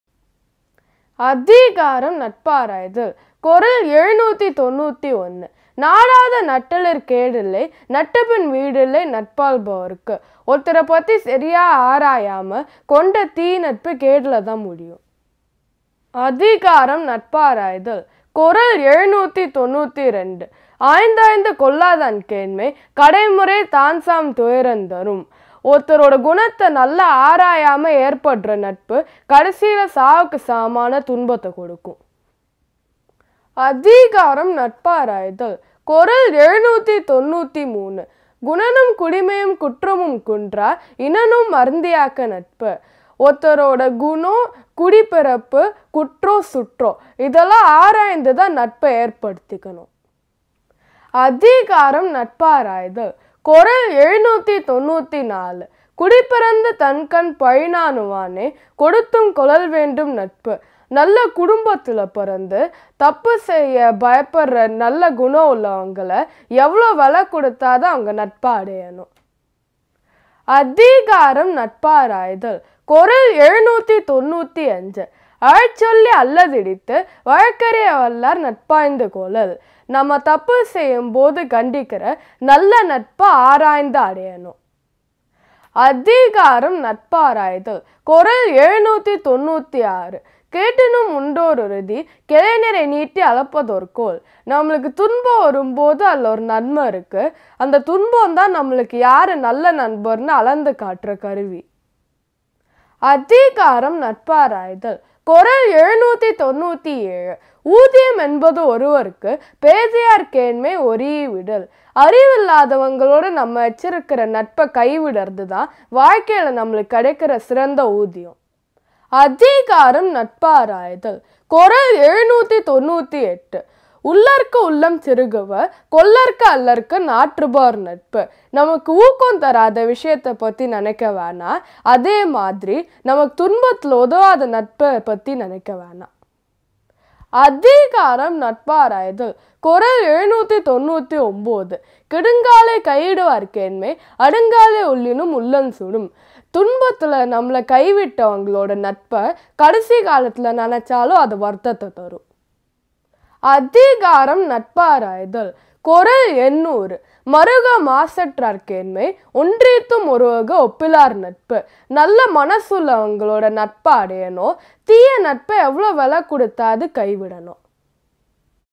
Natparaidal #791 - #800 நட்பாரய்தல் - Thirukkural with a simple meaning
Assessment before entering a friendship

Nothing can be more pernicious than accepting a friend without assessing; after accepting, there is no letting go of them. 791

Assess and assess before taking a friend,
else the ensuing anguish will be fatal. 792

Know one’s traits, roots, faults and flawless
kinship, and then forge a friendship. 793

Of high birth, and fears disgrace –
give whatever and gain the friendship. 794

When you err, they may chide you till you cry,
and show you the right way – assess and accept their friendship. 795

There is a gain in grief: it is a tool
to stretch and gauge a friend. 796

It is a gain to lose
the friendship of a fool. 797

Think not of trifles that stifle your fervor;
take not the friendship of those who desert in distress. 798

A friend who abandons in times of adversity, will torment the heart even at death’s door. 799

Seek friendship with the impeccable; give anything to sever ties with unbefitting allies. 800